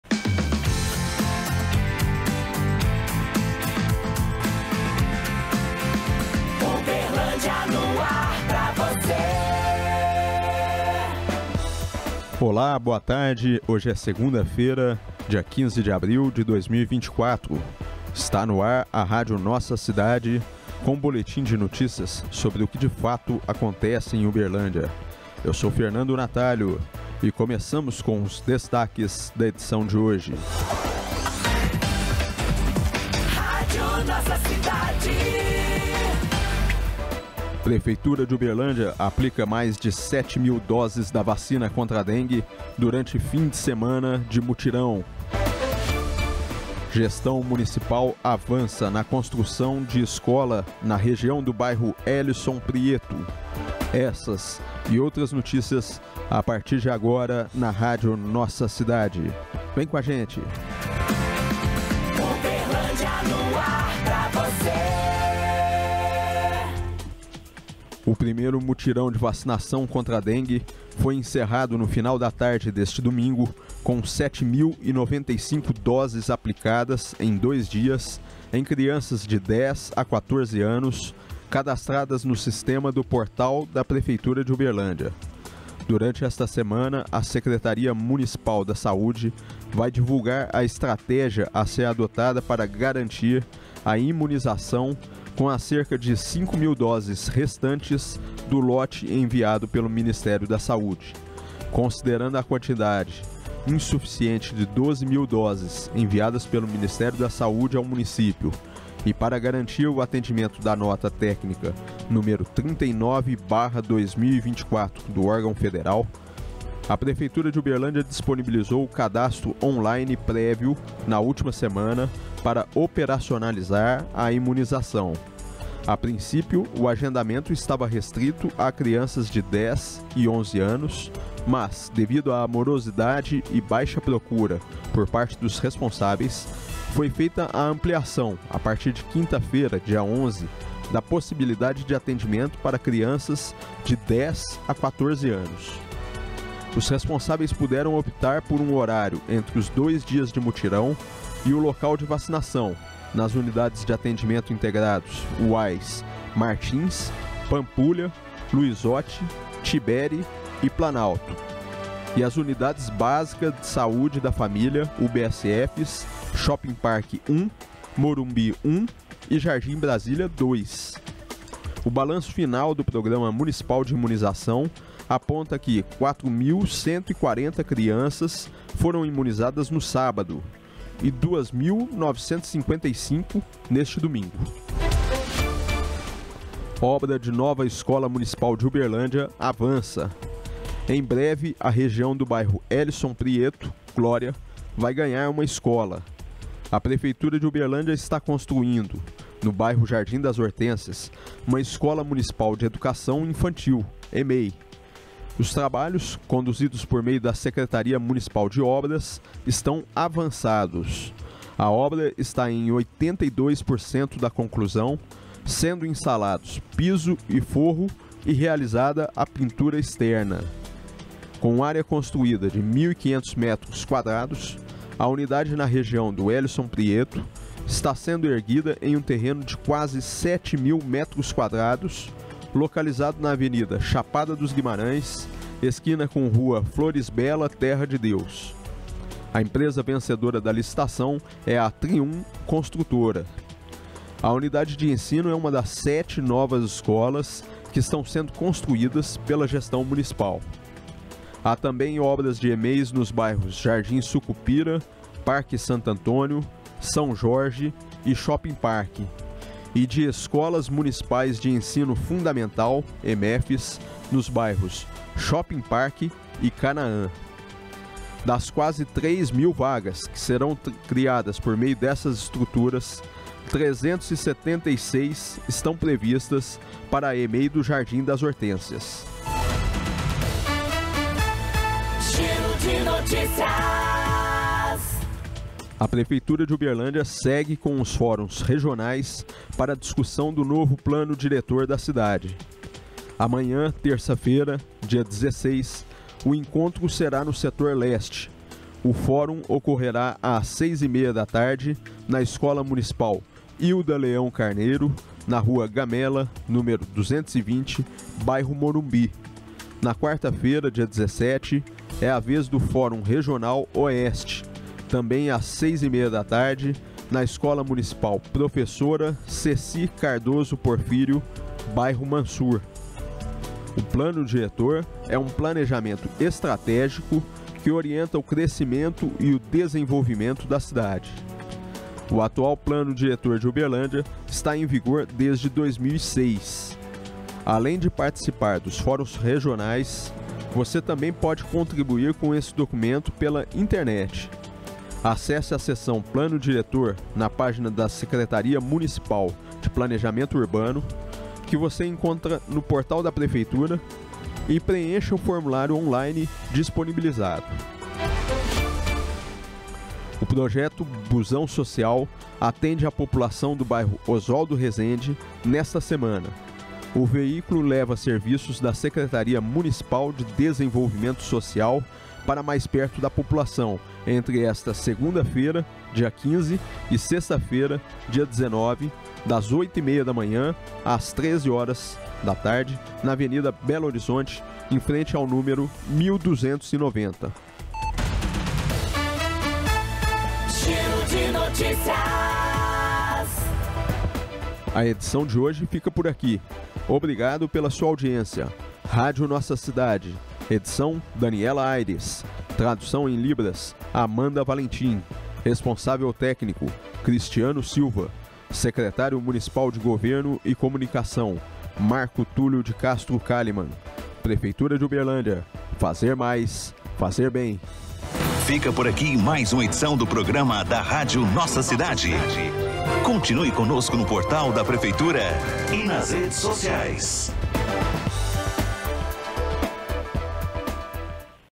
#1166: Prefeitura de Uberlândia aplica 7.095 doses da vacina contra a dengue durante fim de semana
Construção de escola municipal avança na região do bairro Élisson Prieto. E ainda: Amanhã (16) acontece mais um fórum regional para discussão no novo Plano Diretor de Uberlândia.